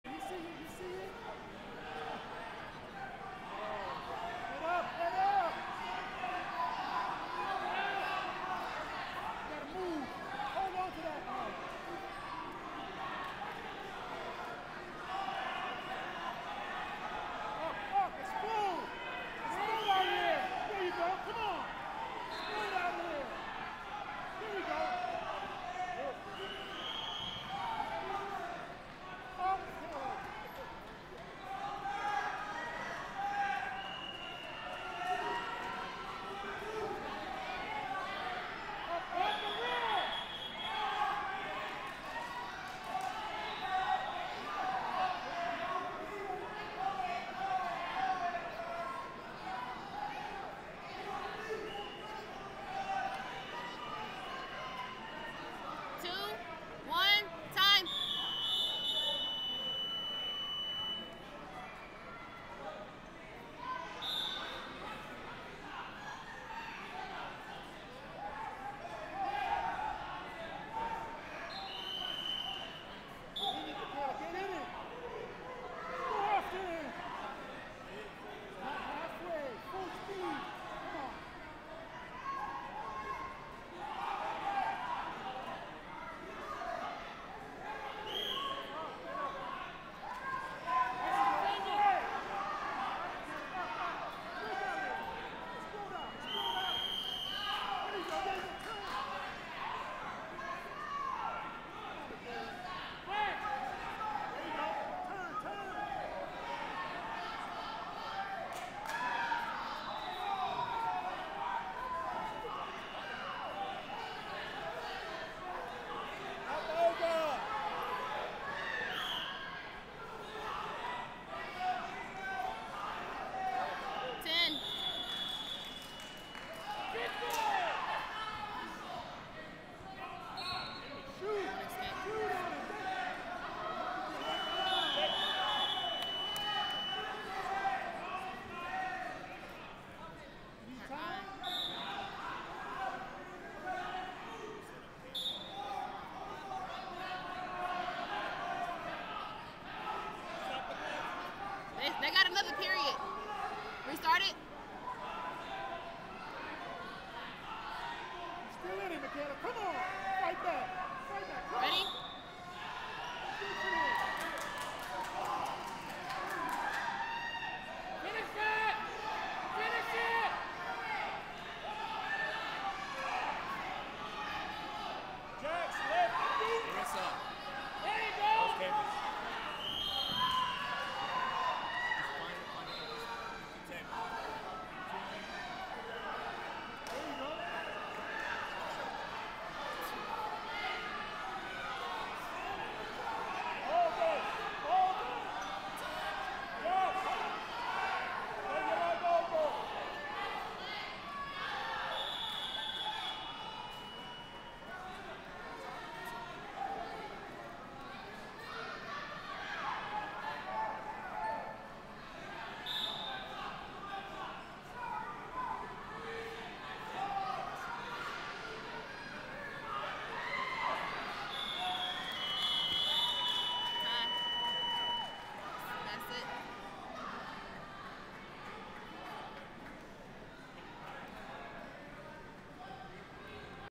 8U 53 Gabriel Jaxon Medrano Gracie Barra Westchase Vs Michaela Brown Ton Best Trained Wrestling
2019 Stockyard Stampede - 8U 53 Gabriel Jaxon Medrano Gracie Barra Westchase Vs Michaela Brown Ton Best Trained Wrestling